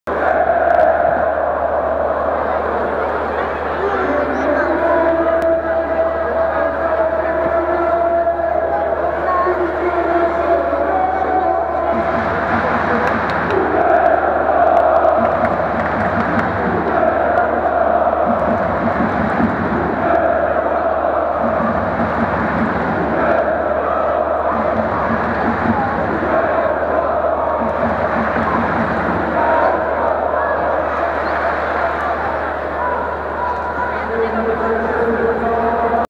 Śląsk (POL) - Paide (EST). Home fans_1
UEFA Europa Conference League
First qualifying round, 2nd leg, Thursday, 15 Jul 2021, 21:00

Śląsk (POL) - Paide (EST)
Kibice/fans/supporters

Stadion Miejski we Wrocławiu